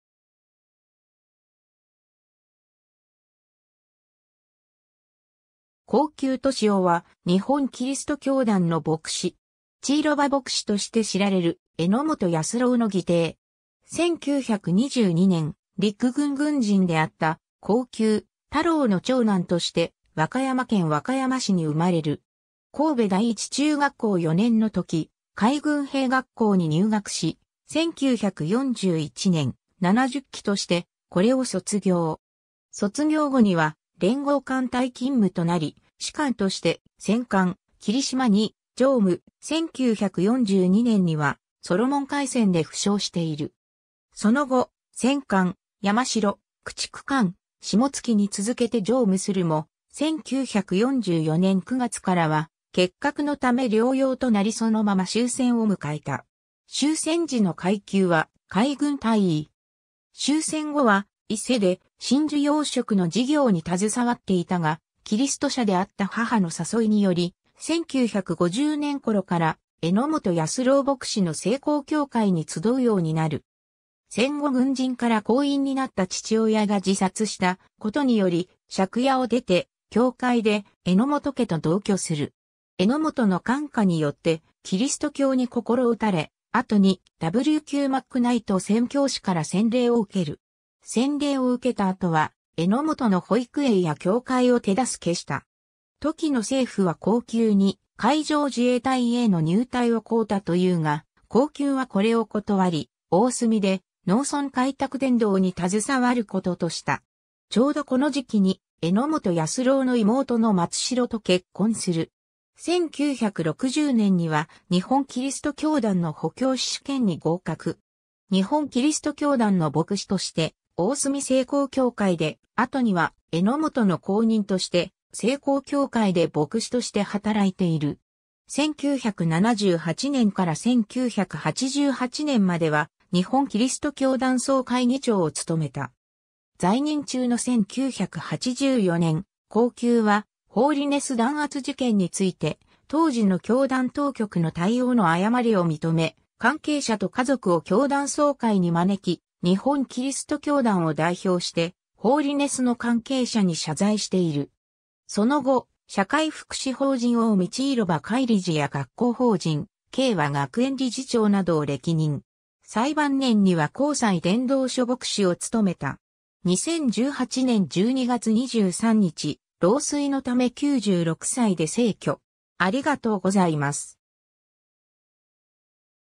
後宮俊夫
後宮 俊夫（うしろく としお、1922年4月12日 - 2018年12月23日）は、日本基督教団の牧師。
「ちいろば牧師」として知られる榎本保郎の義弟。
 1922年、陸軍軍人（陸軍中佐）であった後宮太朗の長男として和歌山県和歌山市に生まれる。
神戸第一中学校4年の時、海軍兵学校に入学し、1941年、70期としてこれを卒業。
卒業後には連合艦隊勤務となり士官として戦艦「霧島」に乗務、1942年にはソロモン海戦で負傷している。
その後、戦艦「山城」、駆逐艦「霜月」に続けて乗務するも、1944年9月からは結核のため療養となりそのまま終戦を迎えた。
終戦時の階級は海軍大尉。
 終戦後は伊勢で真珠養殖の事業に携わっていたが、キリスト者であった母の誘いにより、1950年頃から榎本保郎牧師の世光教会に集うようになる。
戦後軍人から工員になった父親が自殺したことにより借家を出て教会で榎本家と同居する。
榎本の感化によってキリスト教に心打たれ、後にW.Q.マックナイト宣教師から洗礼を受ける。
洗礼を受けた後は、榎本の保育園や教会を手助けした。
 時の政府は後宮に海上自衛隊への入隊を乞うたというが、後宮はこれを断り、大住（現・京田辺市）で農村開拓伝道に携わることとした。
ちょうどこの時期に榎本保郎の妹の松代と結婚する。
1960年には日本基督教団の補教師試験に合格。
日本基督教団の牧師として大住世光教会で、後には榎本の後任として世光教会で牧師として働いている。
 1978年から1988年までは日本基督教団総会議長を務めた。
在任中の1984年、後宮はホーリネス弾圧事件について、当時の教団当局の対応の誤りを認め、関係者と家族を教団総会に招き、日本基督教団を代表してホーリネスの関係者に謝罪している。
 その後、社会福祉法人近江ちいろば会理事や学校法人敬和学園理事長などを歴任。
 最晩年には甲西伝道所牧師を務めた。
 2018年12月23日、老衰のため96歳で逝去。